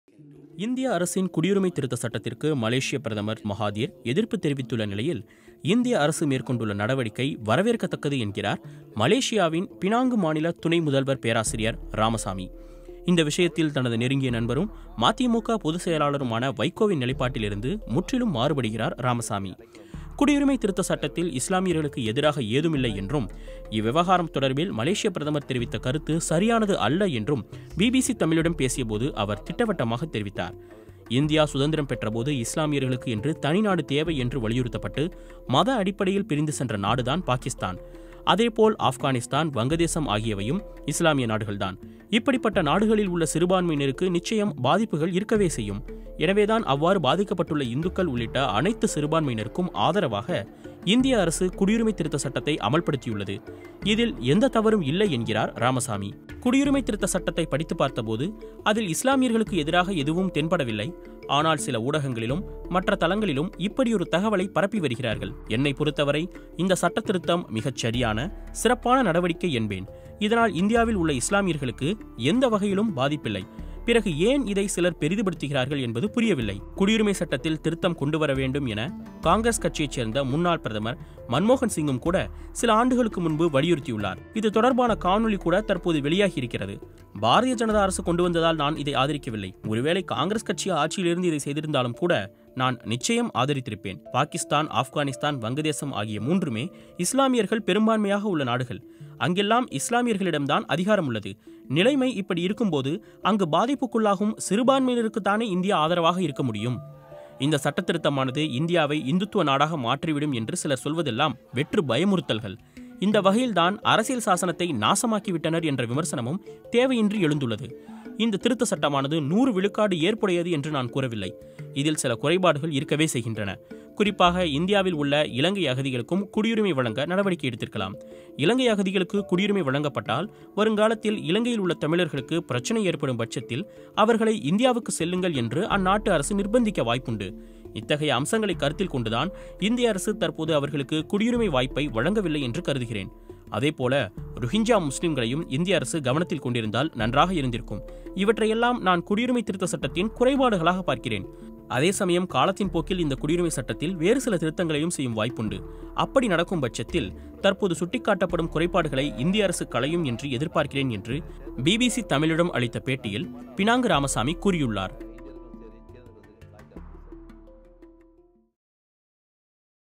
CAA முஸ்லிம்களுக்கு எதிரானது அல்ல. Malaysia PM கருத்து சரியில்லை - Penang Ramasamy
குடியுரிமை திருத்த சட்டம் இஸ்லாமியர்களுக்கு எதிரானது அல்ல: பினாங்கு துணை முதல்வர் ராமசாமி

இந்திய அரசின் குடியுரிமை திருத்த சட்டத்திற்கு மலேசியப் பிரதமர் துன் மகாதீர் எதிர்ப்பு தெரிவித்துள்ள நிலையில், இந்திய அரசு மேற்கொண்டுள்ள நடவடிக்கை வரவேற்கத்தக்கது என்கிறார் மலேசியாவின் பினாங்கு மாநில துணை முதல்வர் பேராசிரியர் ராமசாமி.